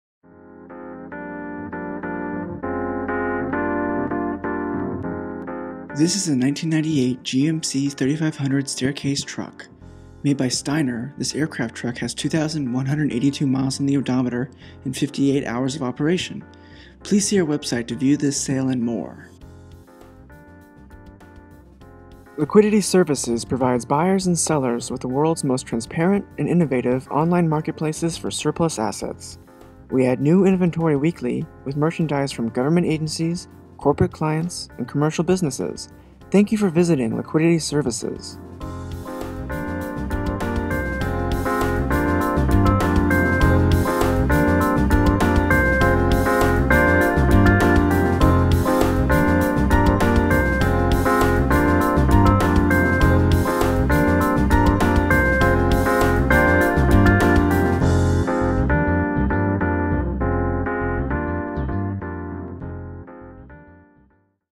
1998 GMC 3500 Staircase Truck on GovLiquidation.com
- This 1998 GMC 3500 Staircase Truck has been sold. This Staircase was located in Travis AFB, CA.

Made by Stinar this aircraft truck has 2,182 miles on the odometer and 58 hours of operation. Please see our website to view this sale and more.

To view a current list of available Trucks and Other Vehicles, today!